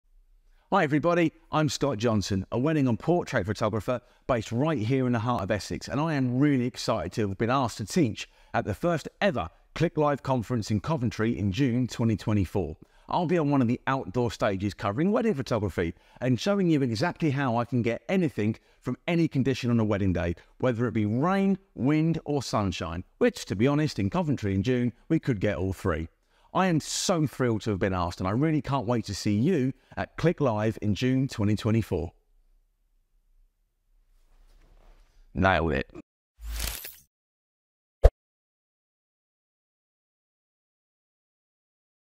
Scott Johnson Educator at Click Live 2024
📸 Hey photography enthusiasts! Dive deep into the world of photography with the incredible Scott Johnson at Click Live 2024 in Coventry, UK. From hands-on workshops, exciting gear showcases, to interactive shooting setups, it's a haven for every shutterbug out there. Plus, get a chance to meet Scott Johnson  in person! 🌟 Mark your calendars and be part of this grand photography event.

🛍️ Secure your spot now and immerse yourself in a world of photography like never before. Book now!

Transcript